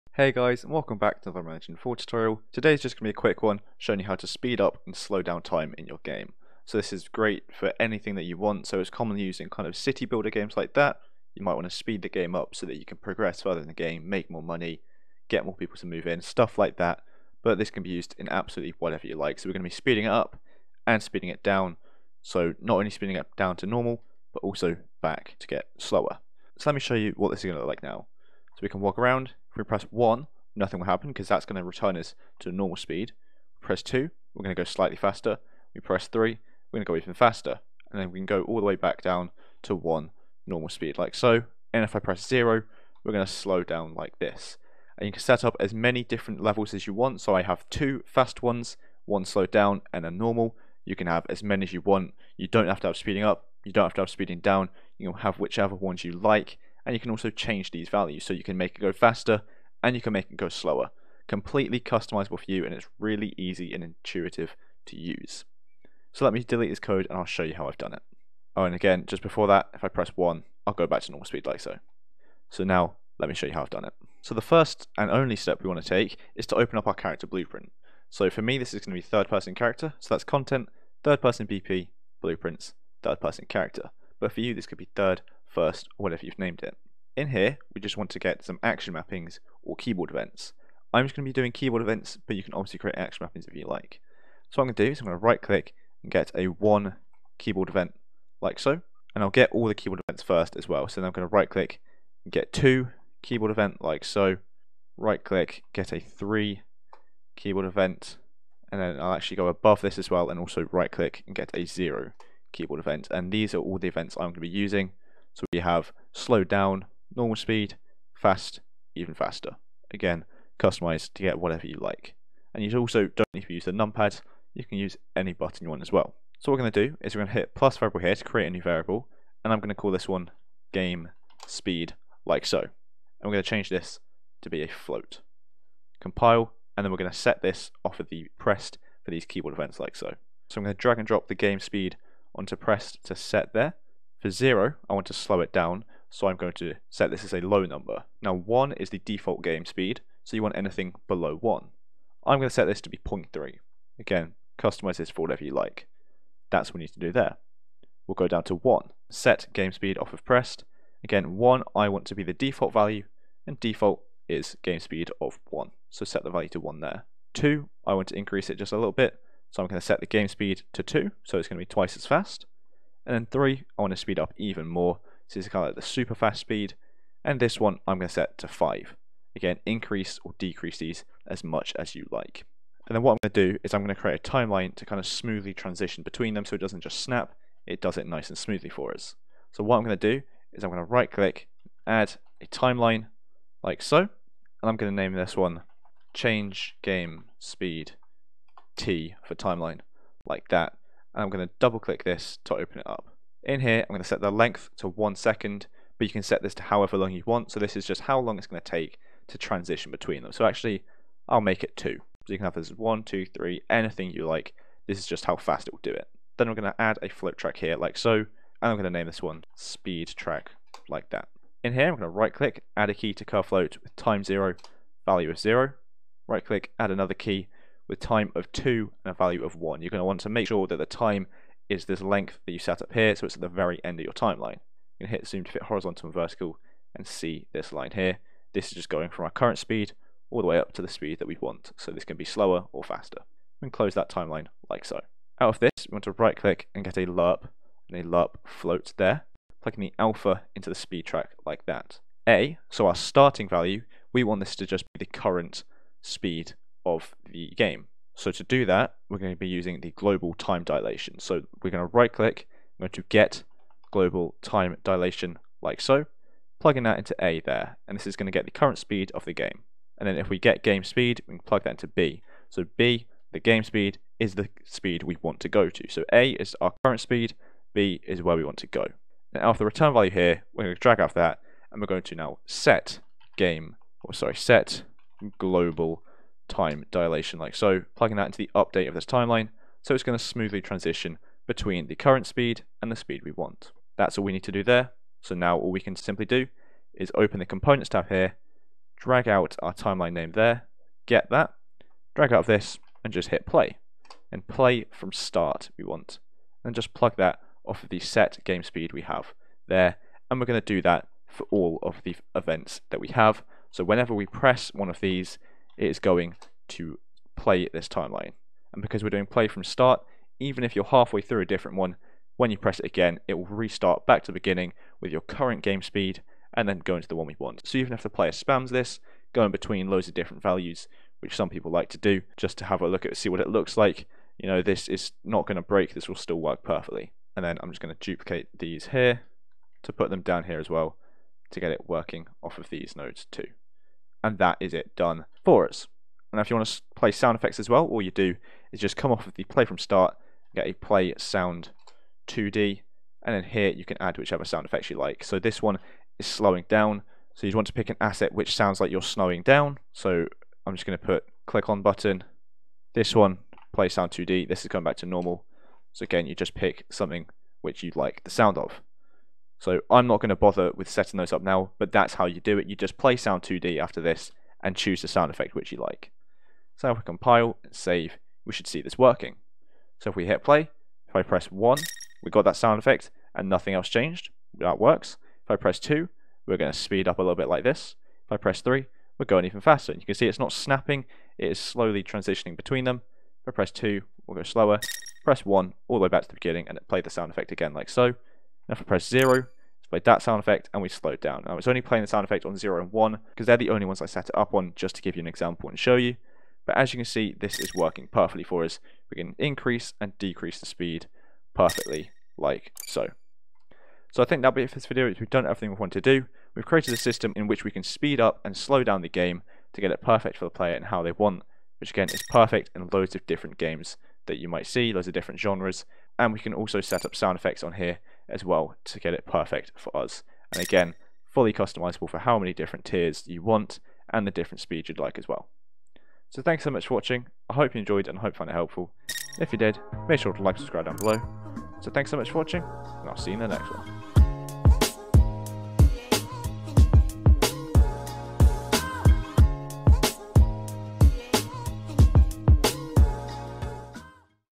How To Speed Up And Slow Down Time In Game - Unreal Engine 4 Tutorial
Hey guys, in today's video, I'm going to be showing you how to speed up and slow down time in your game in real time. We can do this by a simple push of a button or anyway you like, and it can snap or be a smooth transition between the speeds.

00:00 - Intro
00:26 - Overview
01:14 - Tutorial
06:54 - Final Overview
07:16 - Outro